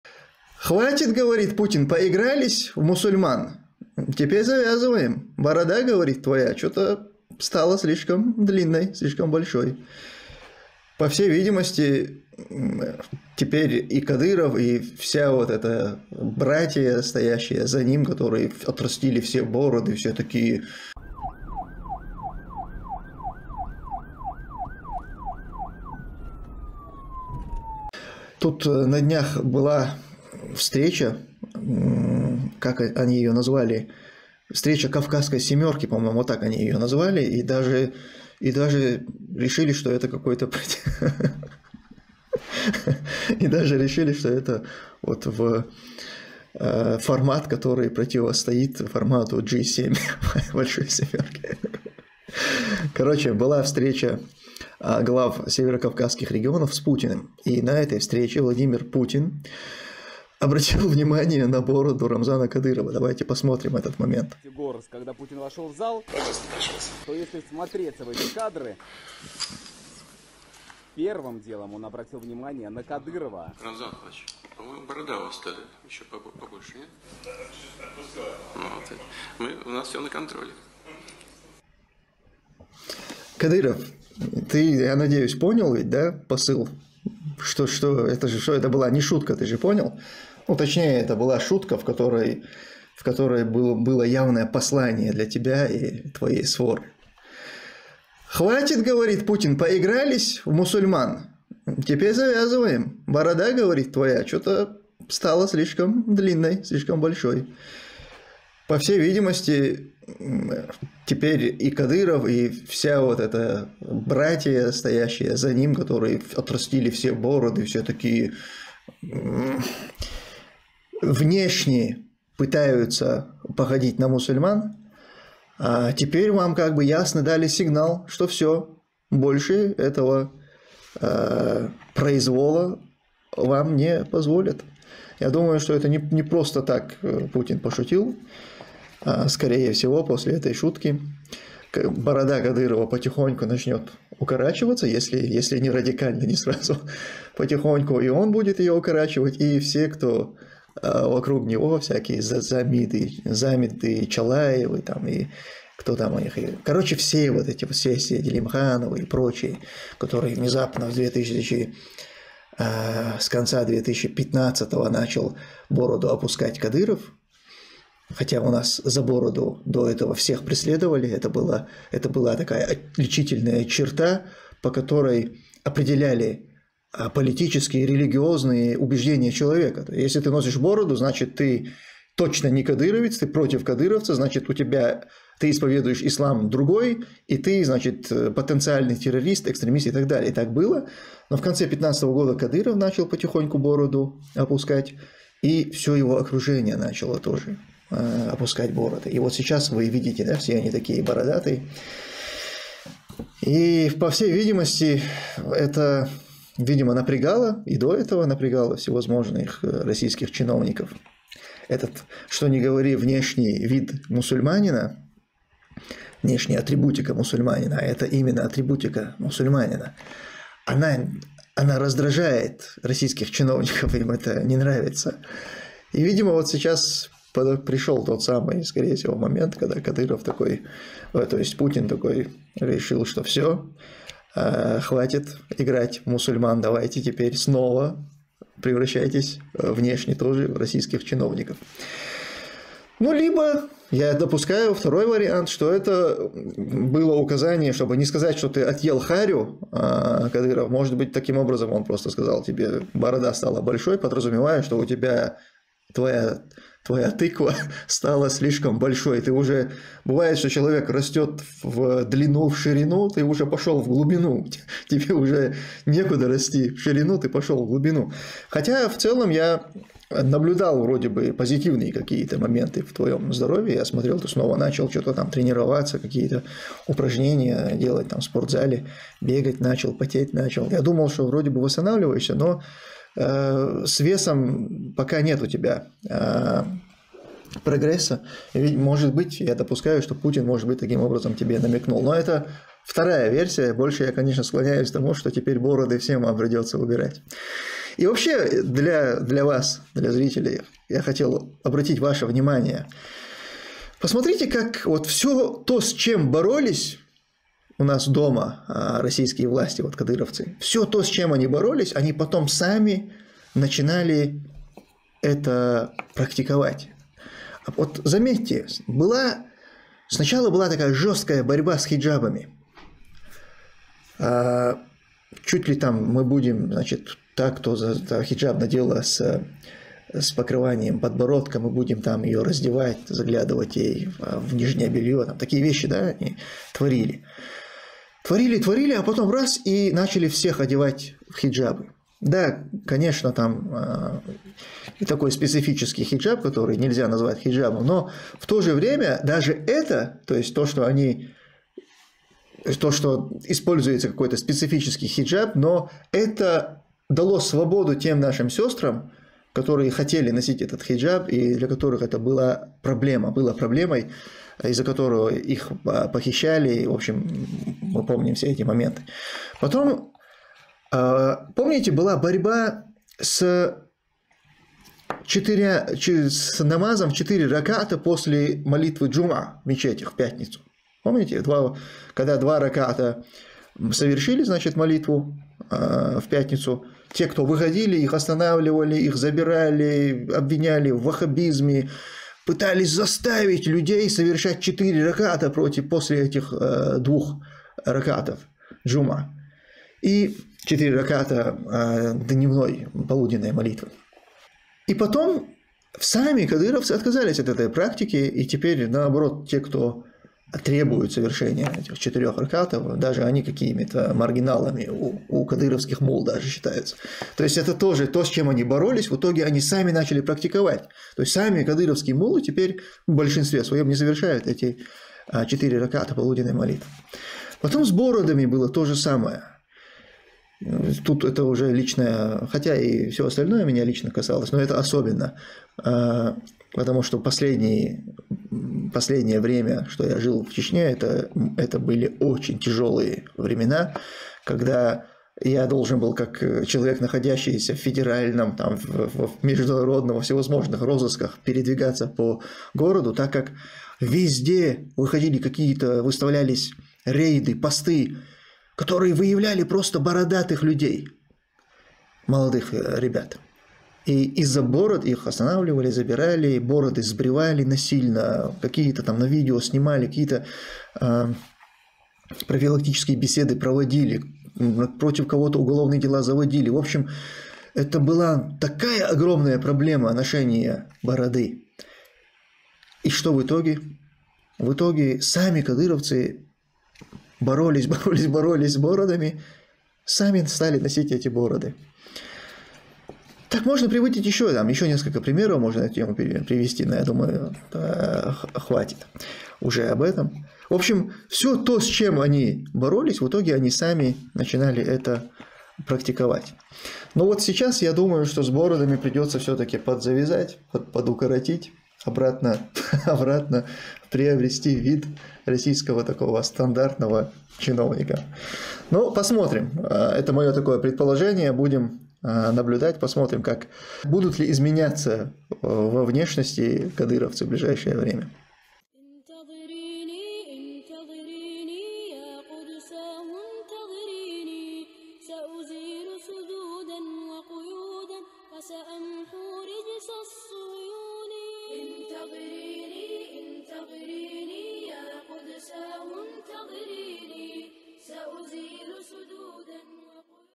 КАДЫРОВСКАЯ БОРОДА РАЗДРОЖАЕТ ПУТИНА
КАКОЙ ВАРИАНТ ВЫБИРАЕТЕ ВЫ?  1 - КАДЫРОВ СБРОЕТ БОРОДУ;  2 - КАДЫРОВ УКОРОТИТ  БОРОДУ;  3 - КАДЫРОВ НЕ ОТРЕАГИРУЕТ НА ЗАМЕЧАНИЕ ПУТИНА: 4 - СВОЙ ВАРИАНТ.